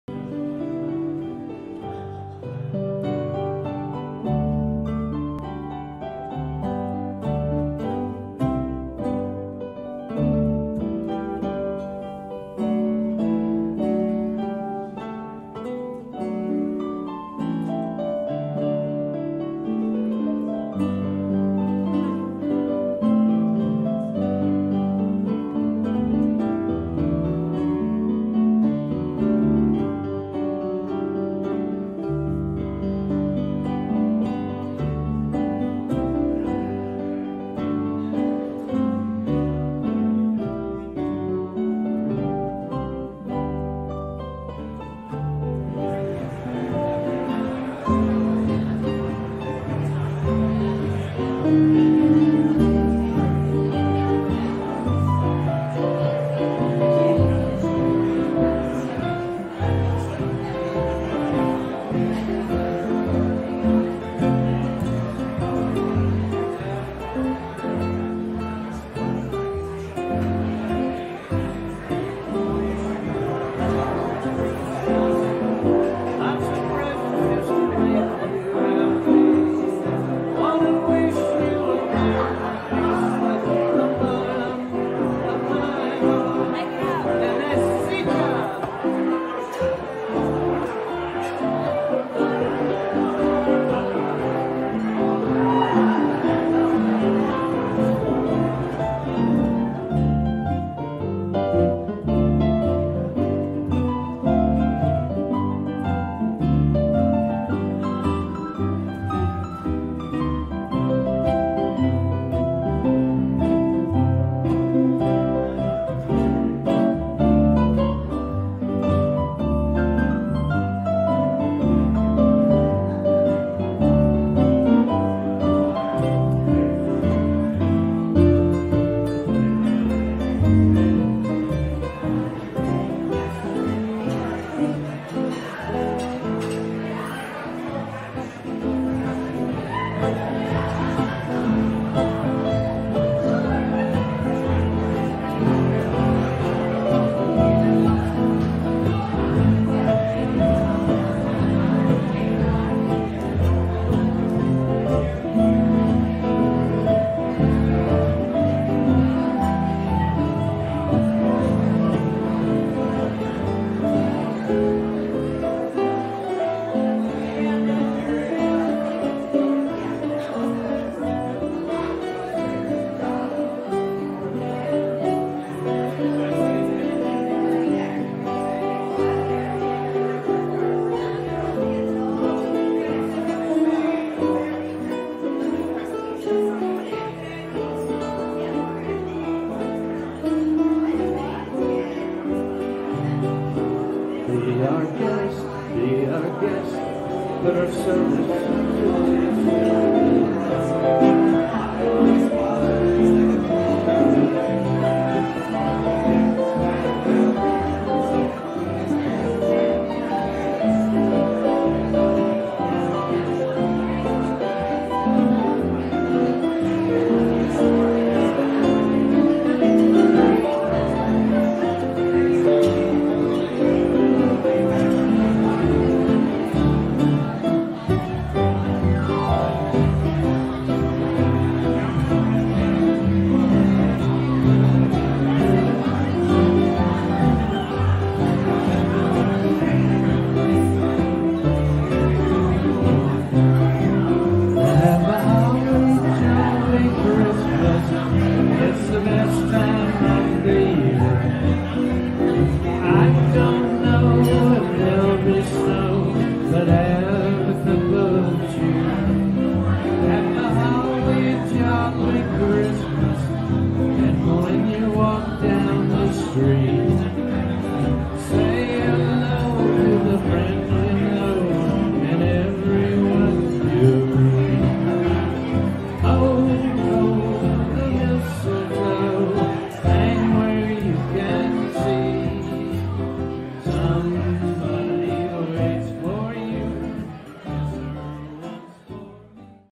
Brighton Street Band... Christmas Dance Destin Hilton Fred Astaire Dance Club
Saturday Dec 12, 2020... The Brighton Street Band performed for the local Fred Astaire Dance Club... Mostly Christmas Music...